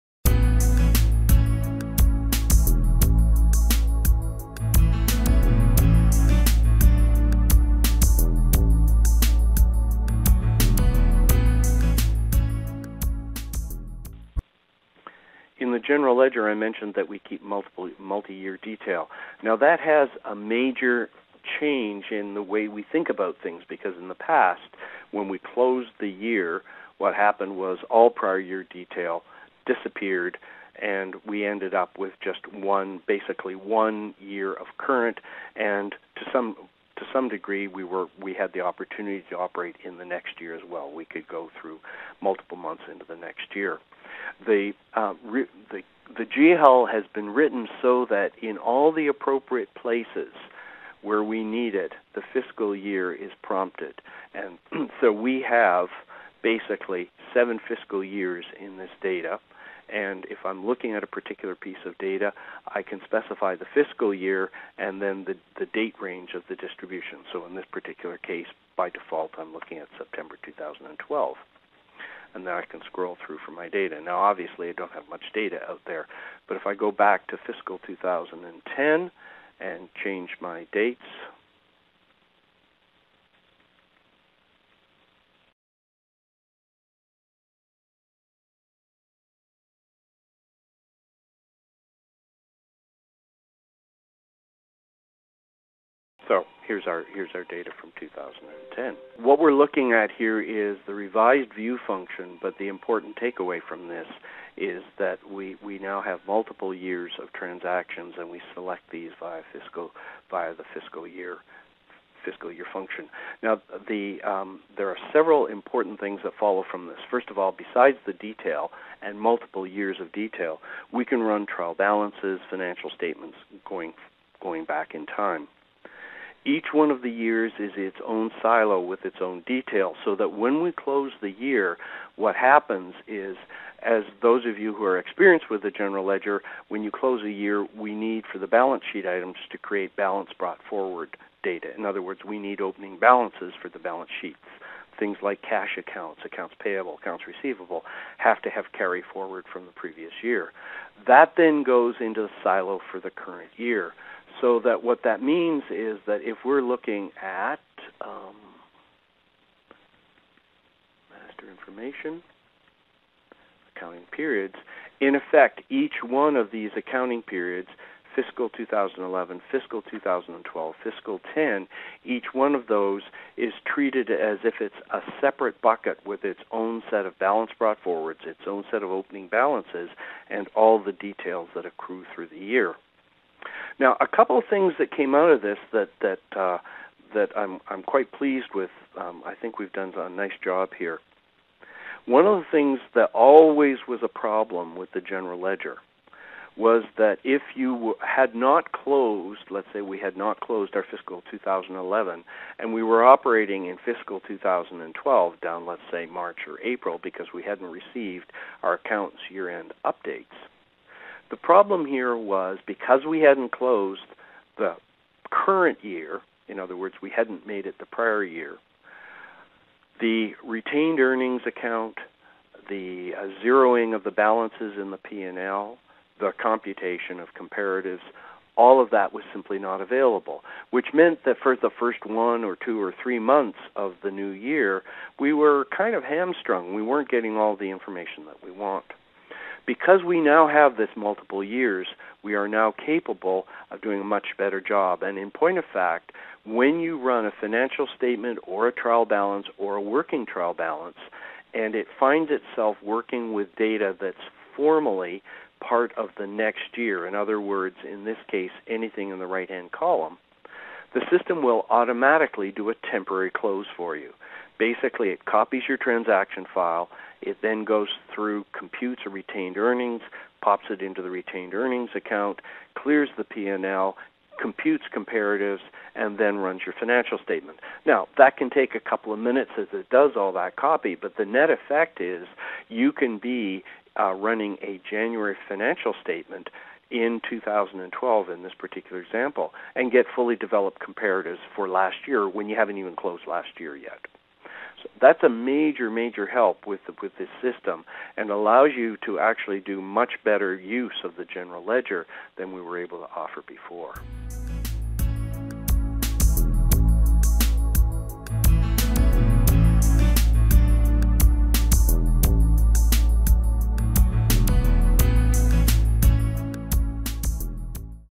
PBS™ Multi Year General Ledger Accounting Software
Passport Business Solutions' General Ledger module will allow you to run reports and even financial statements for your current fiscal year, as well as prior years.

This means you can re-generate statements for any reason well into the past. In this video, Peter Dalziel shares how this works, and why it's such a powerful tool for any business running Passport Business Solutions, looking for a migration path for RealWorld Accounting, or considering a new accounting system all together.

"In the General Ledger I mentioned that we keep multi-year detail. Now that has a major change in the way we think about things because in the past when we closed the year, what happened was all prior year detail disappeared, and we ended up with just basically one year of current, and to some degree we had the opportunity to operate in the next year as well.

We could go through multiple months in to the next year. The GL has been written so that in all the appropriate places where we need it the fiscal year is prompted, and so we have basically seven fiscal years in this data, and if I’m looking at a particular piece of data, I can specify the fiscal year, and then the date range of the distribution, so in this particular case, by default, I’m looking at September 2012.
And then I can scroll though for my data. Now obviously I don’t have much data out there, but if I go back to fiscal 2010 and change my dates, here’s our data from 2010.

What we’re looking at there is the revived view function, but the important takeaway from this is that we now have multiple years of transactions, and we select these via the fiscal year function.
Now there are several important things that follow from this, first of all besides the detail, and multiple years of detail, we can run trial balances, financial statements going back in time.

Each one of the years is its own silo with its own details, so that when we close the year what happens is that those of you who are experienced with the general ledger, when you close the year, we need for the balance sheet items to create balance brought forward data, in other words we need opening balances for the balance sheets."

Passport Software, Inc.
181 Waukegan Rd #200
Northfield, IL 60093